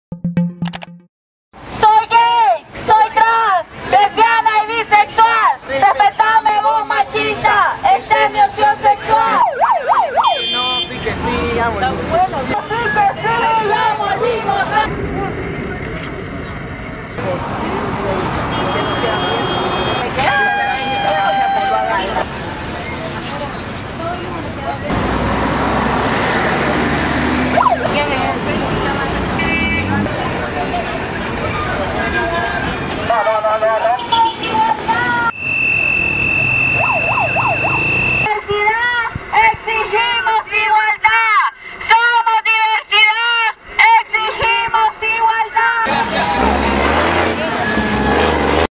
Diversidad sexual de Nicaragua marcha contra la discriminación
Managua, 17 may (EFE), (Imagen: Renee Lucía Ramos).- Decenas de nicaragüenses, en su mayoría homosexuales y activistas, marcharon hoy por una avenida de Managua contra la discriminación por razones de orientación sexual e identidad de género.